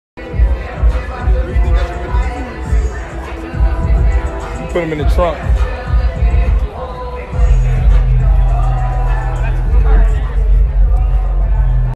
The Craziest Car Encounters in Las Vegas 😱
Las Vegas mandalay bay beach club car set up was fun 🤩